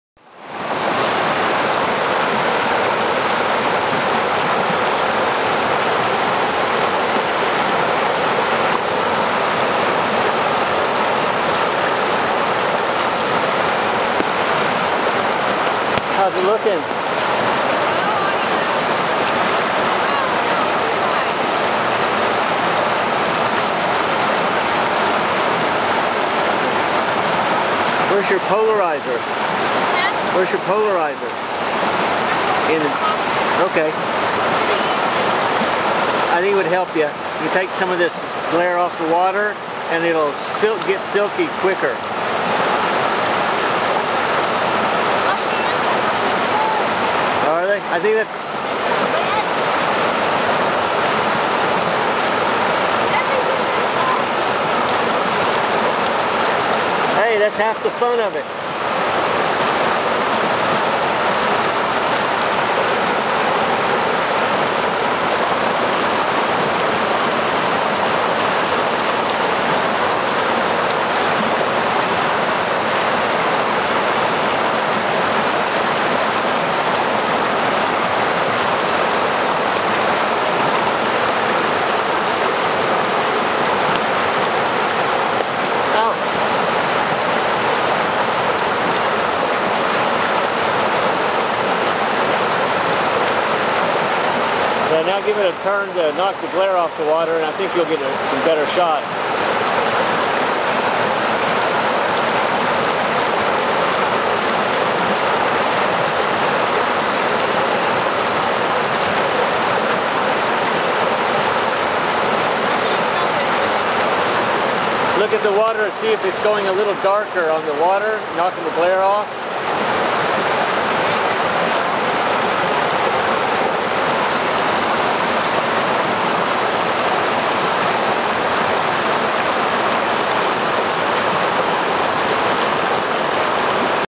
Along the River
Christine and I shooting the Fall color along a river on a rainy day in the Great Smoky Mountains National Park.